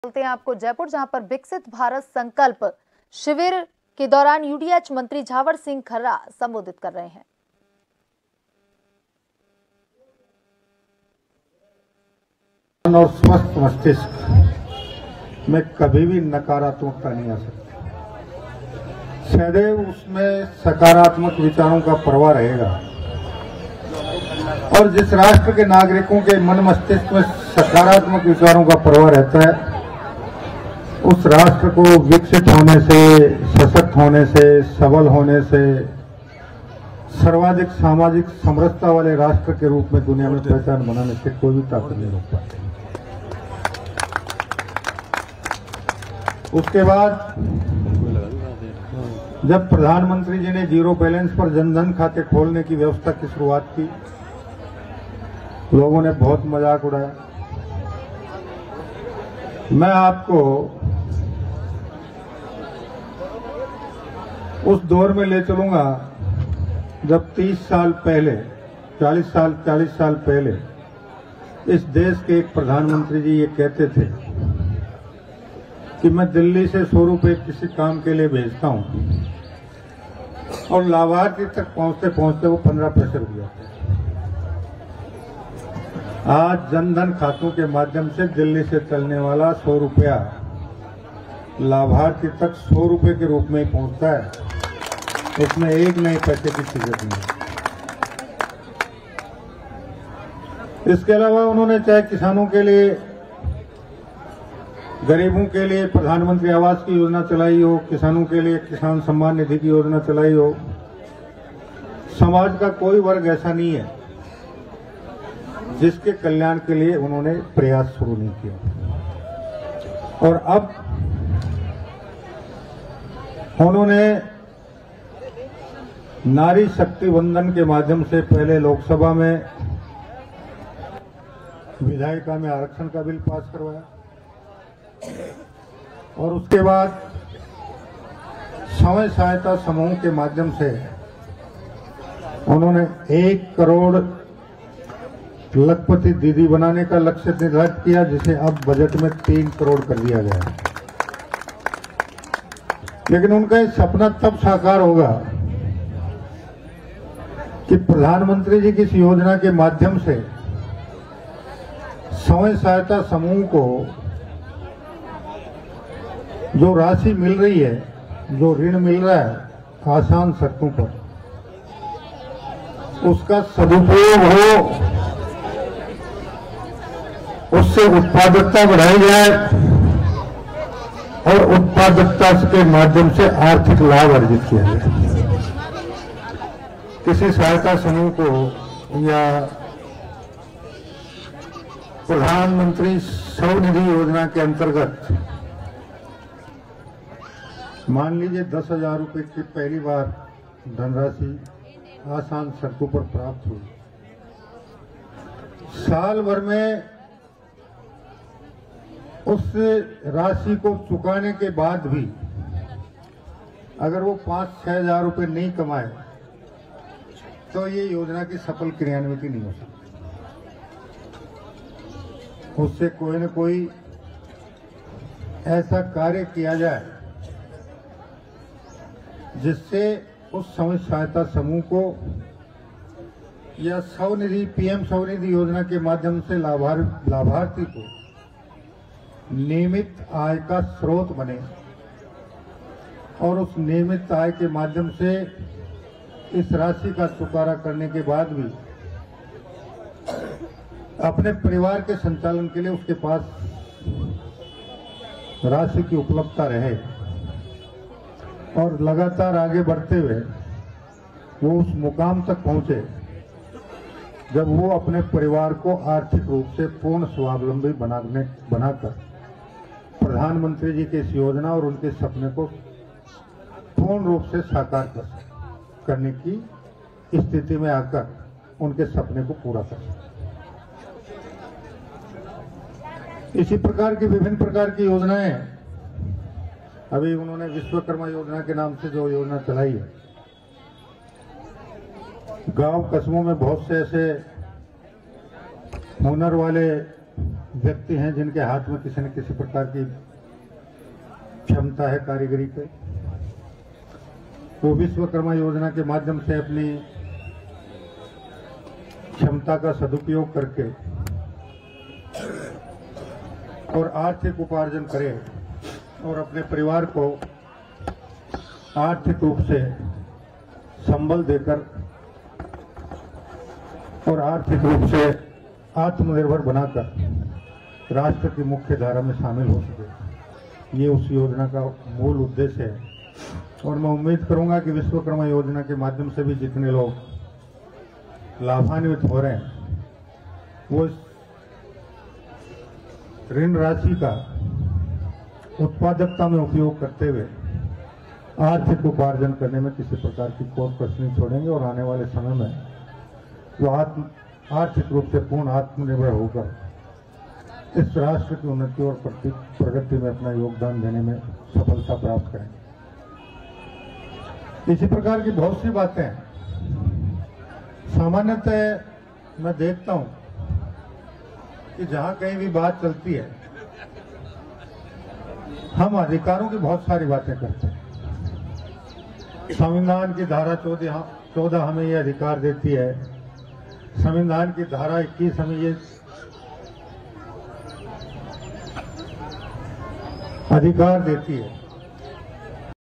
Jaipur Live | विकसित भारत संकल्प शिविर का आयोजन, यूडीएच मंत्री झाबर सिंह खर्रा का संबोधन | JAN TV
Watch JAN TV on :
Tata Play DTH : 1185
Airtel DTH: 355
JIO Fiber: 1384

News Credit-MKP